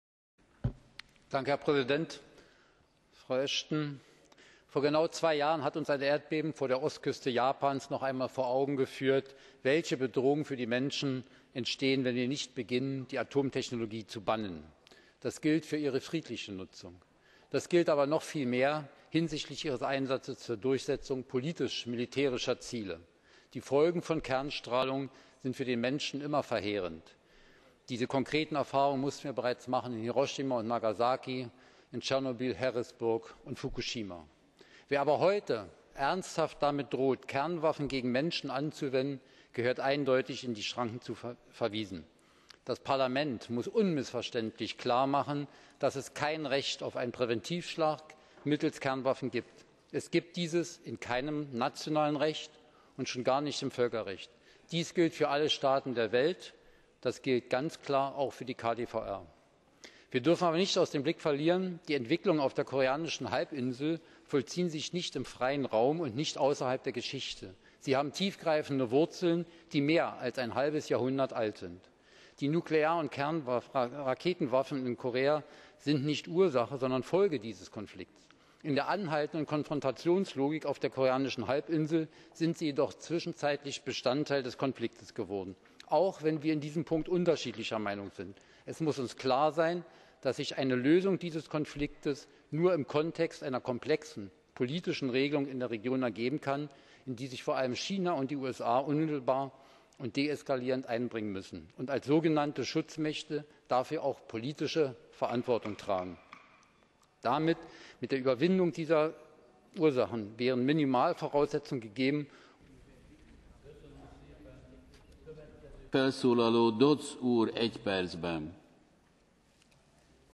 Nukleare Bedrohung und Menschenrechte in Nordkorea
Helmut Scholz, handelspolitischer Sprecher der LINKEN im Europäischen Parlament, im Plenum während der Debatte zu nuklearer Bedrohung in Nordkorea: "Vor genau zwei Jahren hat uns ein Erdbeben vor der Ostküste Japans noch einmal vor Augen geführt, welche Bedrohungen für die Menschen entstehen, wenn wir nicht beginnen, die Atomtechnologie zu bannen. Das gilt für ihre friedliche Nutzung. Das gilt aber noch viel mehr hinsichtlich ihres Einsatzes zur Durchsetzung politisch-militärischer Ziele. Die Folgen von Kernstrahlung sind für den Menschen immer verheerend! Diese konkreten Erfahrungen mussten wir bereits machen -- in Hiroshima und Nagasaki, in Tschernobyl, Harrisburg und Fukushima.

Wer aber heute ernsthaft damit droht, Kernwaffen gegen Menschen einzusetzen, gehört eindeutig in die Schranken verwiesen. Das Parlament muss unmissverständlich klar machen, dass es kein Recht auf einen Präventivschlag mittels Kernwaffen gibt. Es gibt dieses in keinem nationalen Recht und schon gar nicht im Völkerrecht. Dies gilt für alle Staaten der Welt. Das gilt ganz klar auch für die KDVR.

Wir dürfen aber nicht aus dem Blick verlieren, dass sich die Entwicklungen auf der koreanischen Halbinsel nicht im freien Raum und nicht außerhalb der Geschichte vollziehen. Sie haben tiefgreifende Wurzeln, die mehr als ein halbes Jahrhundert alt sind. Die nuklearen Raketenwaffen in Korea sind nicht Ursache, sondern Folge dieses Konflikts. In der anhaltenden Konfrontationslogik auf der koreanischen Halbinsel sind sie jedoch zwischenzeitlich Bestandteil des Konflikts geworden.

Auch wenn wir in diesem Punkt unterschiedlicher Meinung sind, es muss uns klar sein, dass sich eine Lösung dieses Konflikts nur im Kontext einer komplexen politischen Regelung in der Region ergeben kann, in die sich vor allem China und die USA unmittelbar und deeskalierend einbringen müssen und als so genannte Schutzmächte dafür auch politische Verantwortung tragen. Damit -- mit der Überwindung dieser Ursachen -- wären Minimalvoraussetzungen gegeben, ..."

(Der Präsident entzieht dem Redner das Wort.)